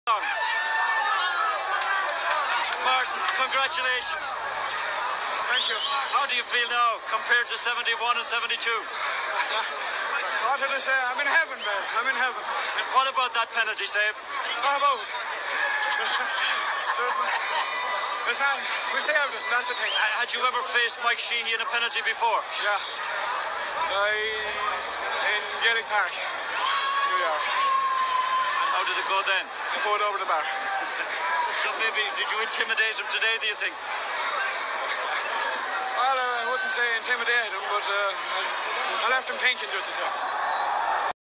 Martin Furlong Interview 1982
After The All Ireland Final 1982